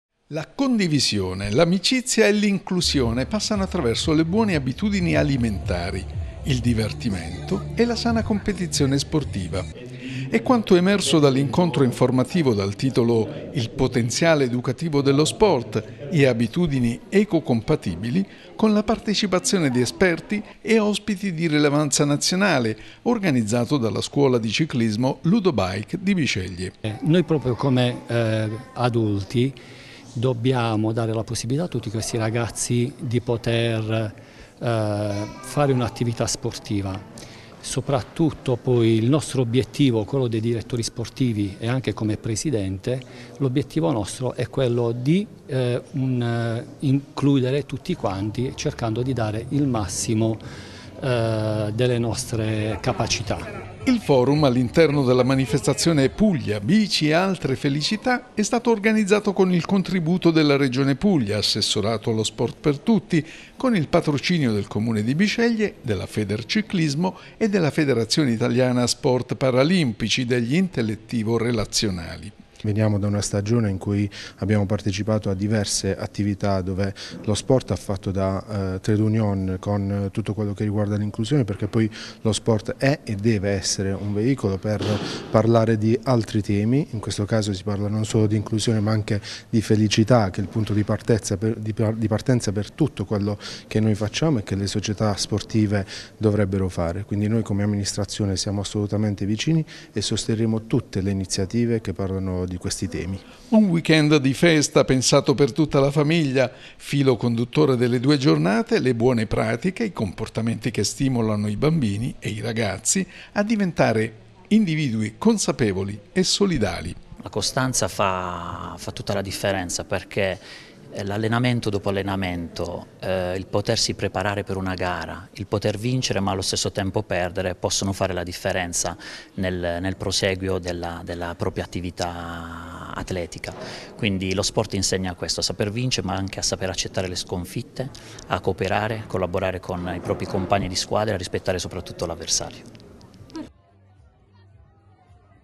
Bisceglie. Un forum informativo sullo sport, la sana alimentazione, l'inclusione
A Bisceglie si è tenuto un forum informativo sullo sport, sulla sana alimentazione e sull’inclusione organizzato dalla Scuola di Ciclismo ASD Ludobike.